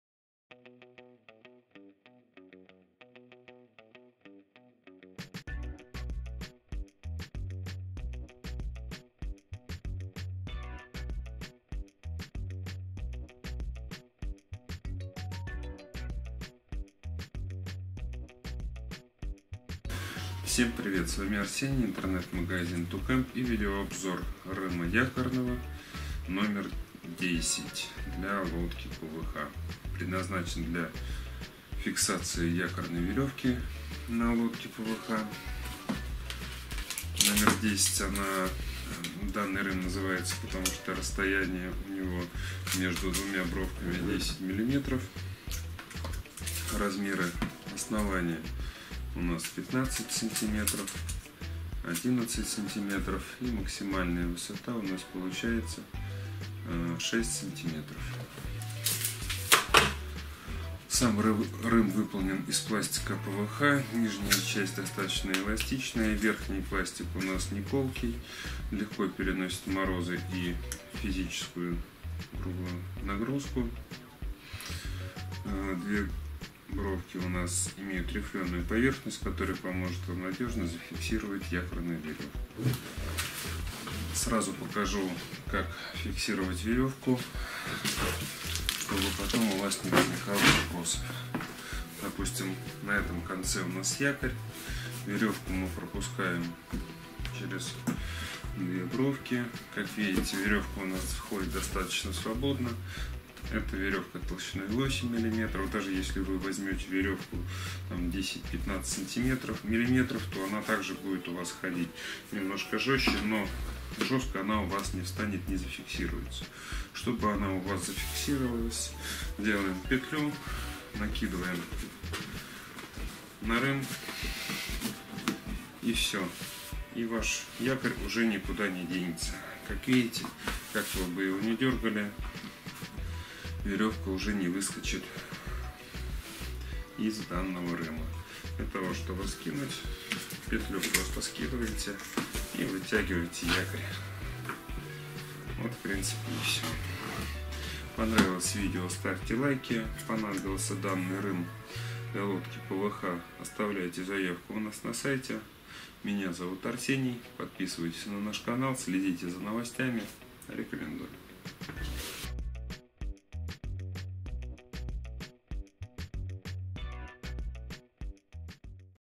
Рым якорный 10
Рым якорный 10 мм поможет вам безопасно спустить и зафиксировать якорь к лодке ПВХ. Данная модель рыма захватит веревку, и если правильно накинуть веревку на рым, то якорь будет вас надежно держать на воде.